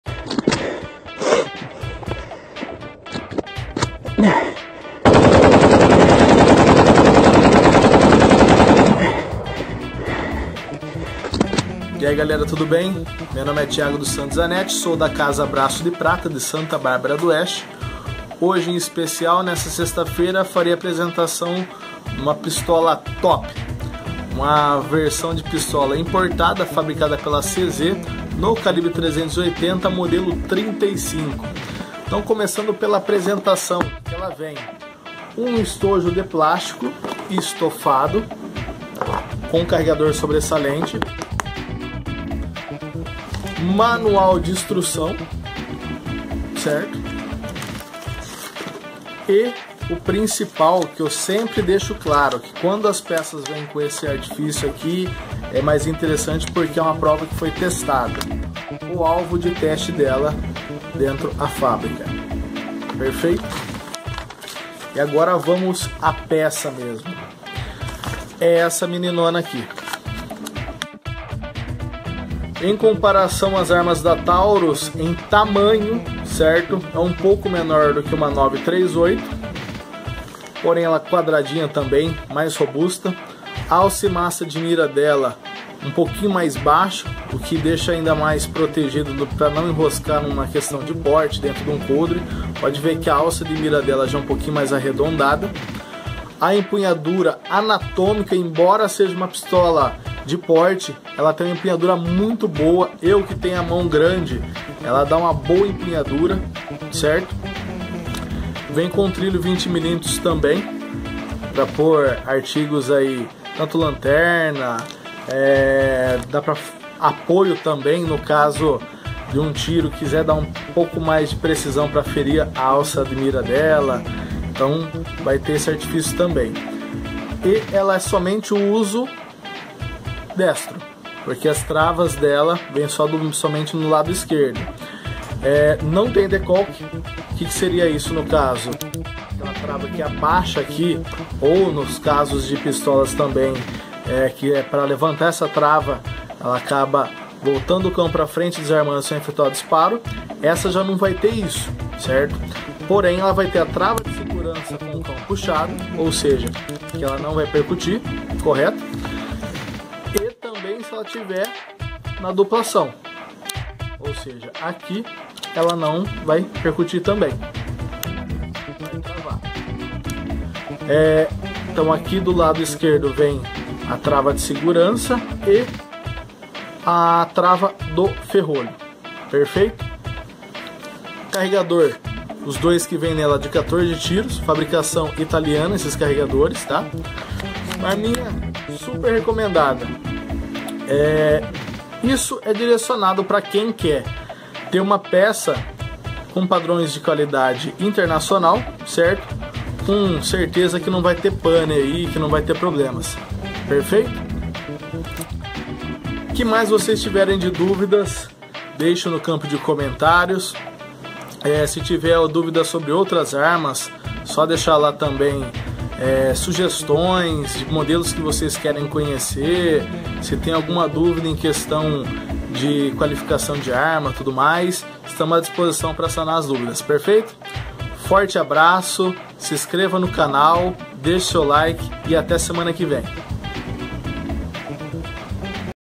Pistola CZ mod. 35 made in Czech Republic
Pistola com padrão de qualidade internacional! CZ 35 no calibre 380 com todos os itens e funcionabilidades apresentados pelo lojista, atirador e amante de armas Thiago Zanetti!